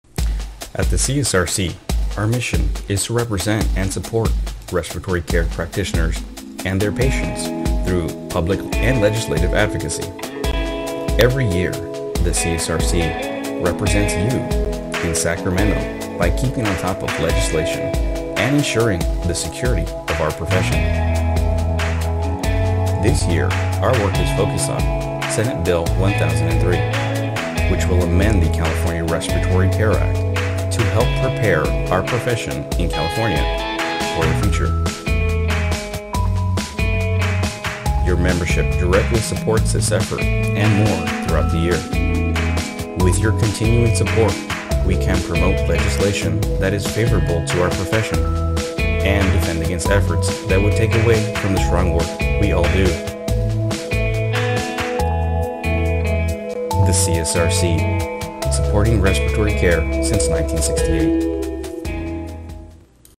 CSRC 2018 Legislative Day
On Tuesday, February 6, 2018, the CSRC visited the California State Capitol to advocate for our profession. Well over 130 RCPs and students attended this year's CSRC Legislative Day.

At this event the CSRC presented Assemblyman Juan Arambula, MD the CSRC Legislator of the Year Award (Picture attached) in a ceremony in the California Assembly Chamber.

Participants attended prearranged meetings with their elected legislators and their staffs to advocate for our profession and to discuss Senate Bill 1003.  SB 1003 will amend the California Respiratory Care Act to allow the Respiratory Care Board to adjust regulations to keep pace with our advancing profession.  The CSRC is co-sponsoring SB 1003 with the RCB and Senator Richard Roth has agreed to shepherd this important legislation through the CA Legislative process.

The CSRC, representing YOU since 1968!